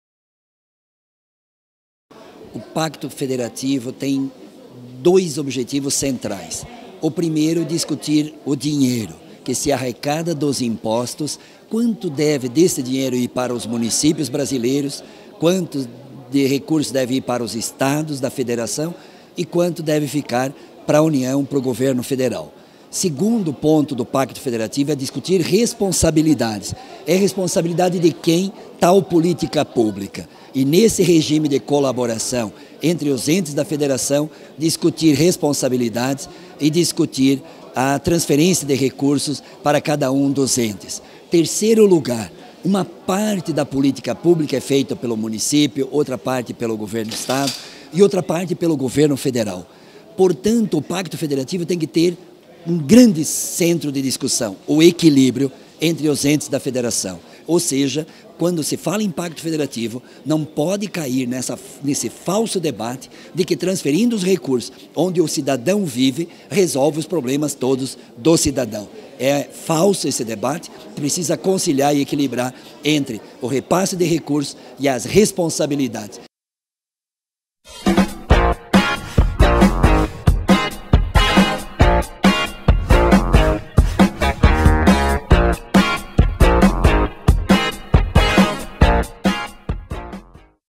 Busca do equilíbrio é o desafio para o Pacto Federativo
Membro da Comissão Especial do Pacto Federativo o deputado Pedro Uczai (PT-SC) analisa o debate em torno do tema e avisa: "é preciso debater não apenas recursos, mas a responsabilidade dos entes pela execução de cada política pública". Confira.